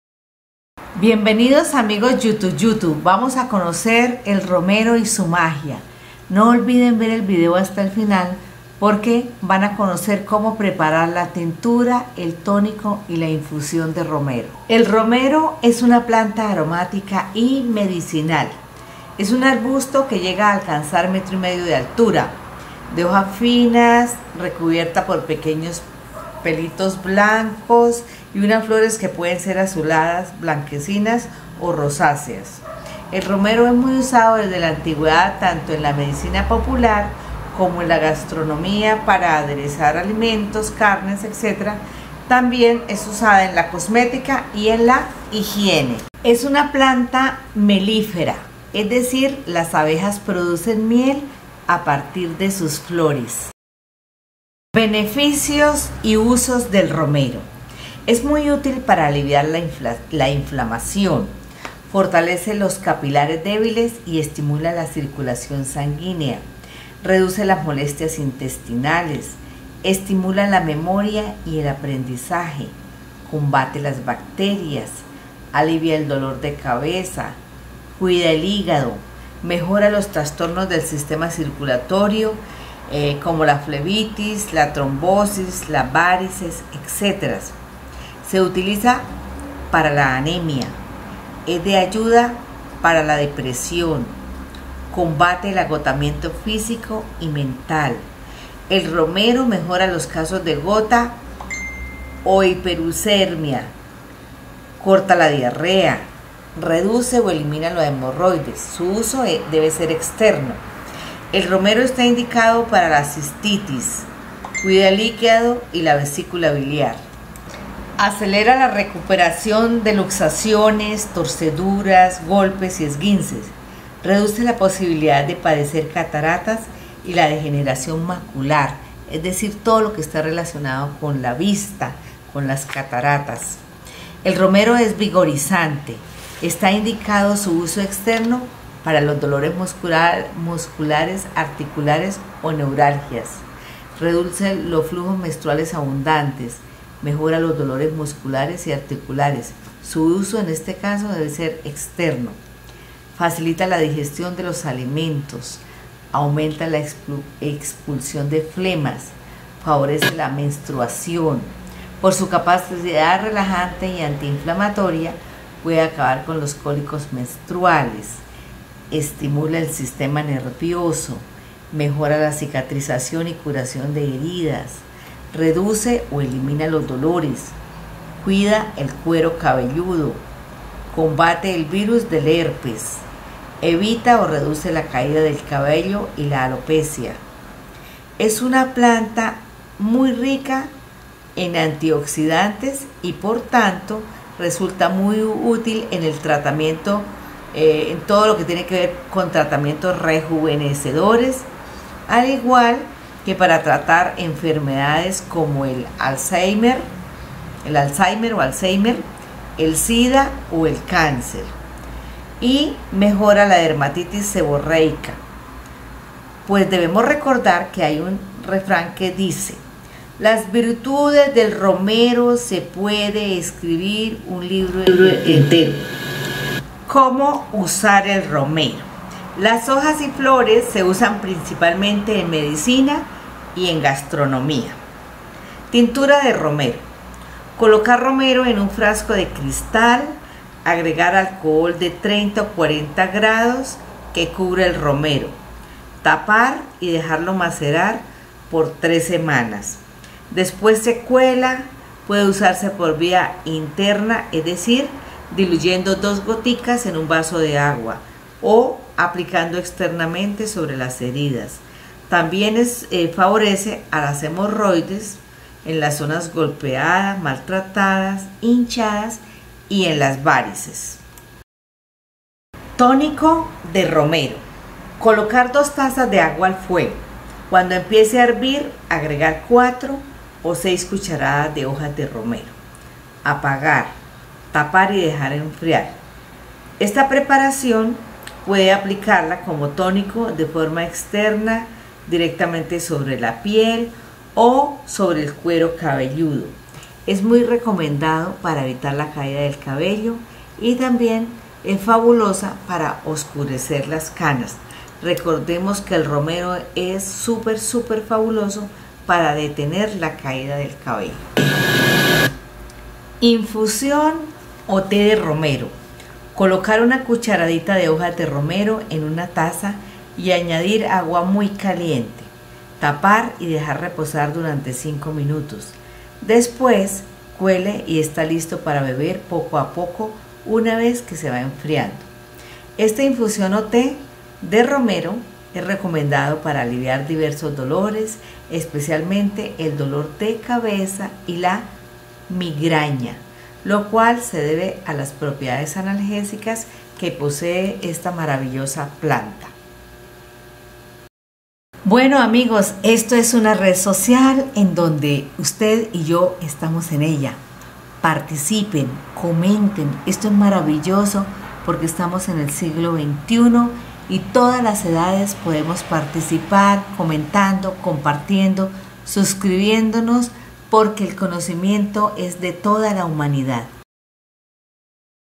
El PODER DEL ROMERO, mejora la memoria, cura el dolor de cabeza, la cistitis y la circulación
El ROMERO mejora la memoria, cura el dolor de cabeza, la cistitis y la circulación sanguínea. Evita la caída del cabello, oscurece las canas, calma los cólicos menstruales, combate el agotamiento, corta la diarrea, reduce la hemorroides. Al final del video conozca como preparar tintura,  infusión o té y tónico de romero.